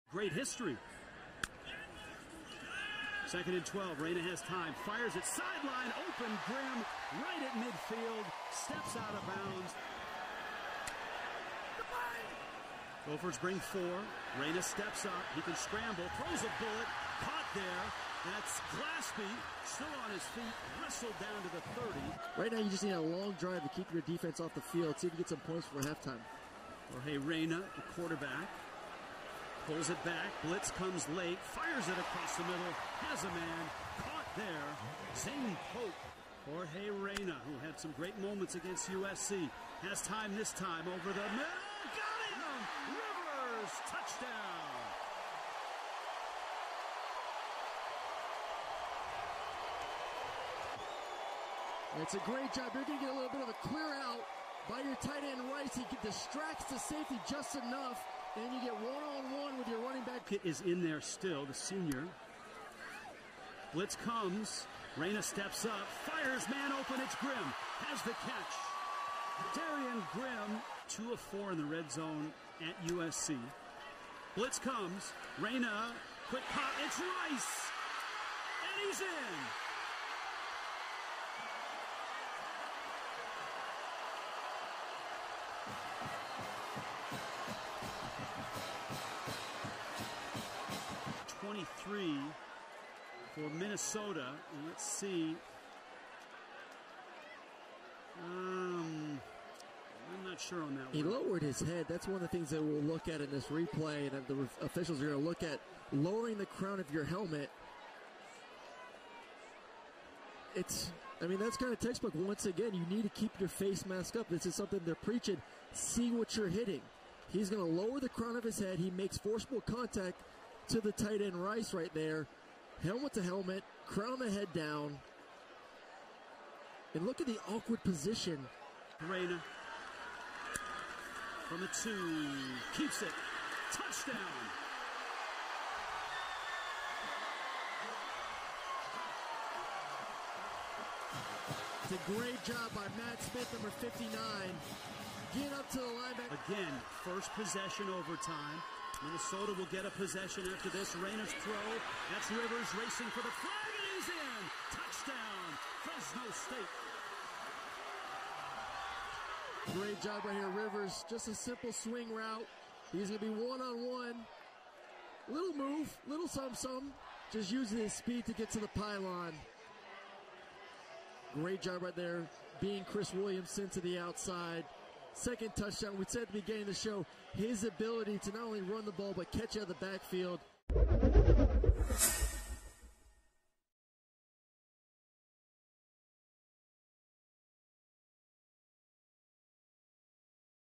Jorge Reyna Highlights: Minnesota vs. Fresno State (2019) | Stadium
Fresno State QB Jorge Reyna passed for 288 yards and 3 scores in a losing effort to Minnesota 38-35 in 2OT on Saturday night. Check out the top plays from the Bulldogs' senior QB in Week 2.